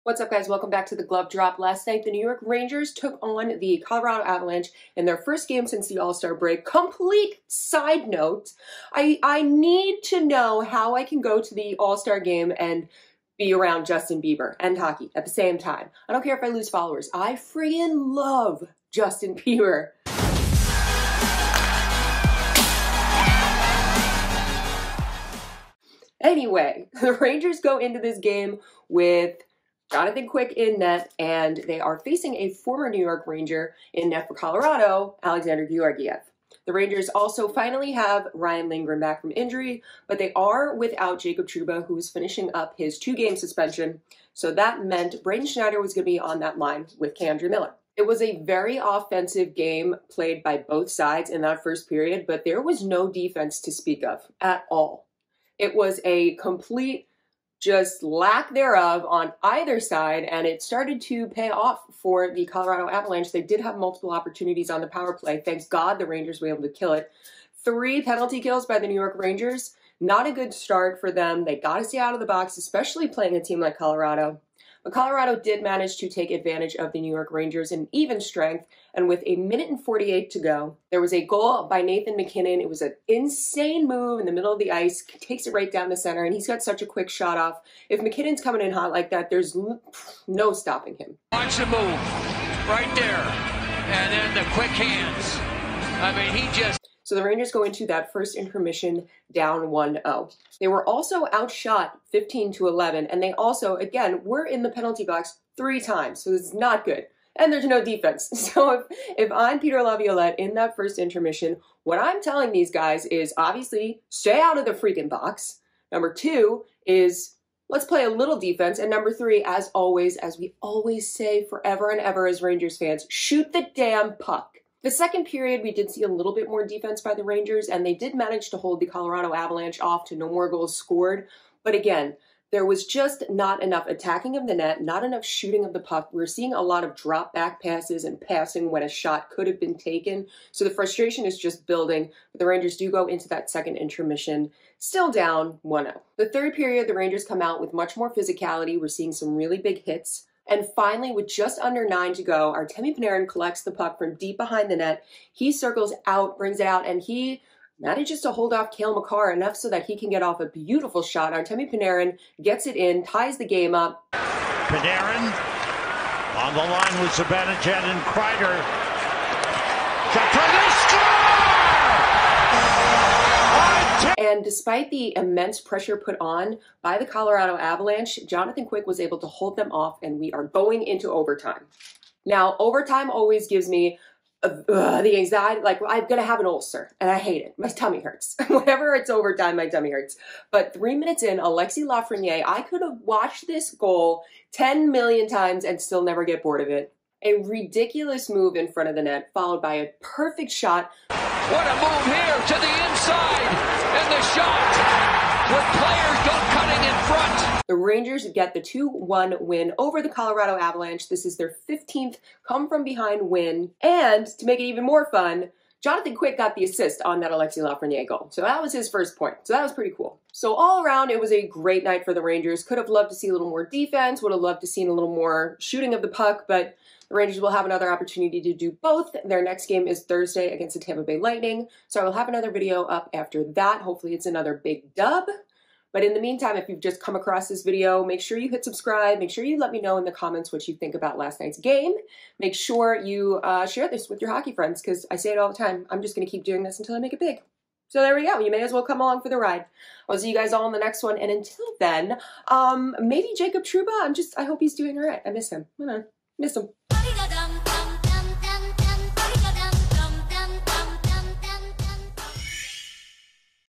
NY Rangers comeback to beat Colorado Avalanche in OT with huge goals by Panarin and Lafreniere
for more of the glove drop check me out on instagra, tik tok, and x @TheGloveDrop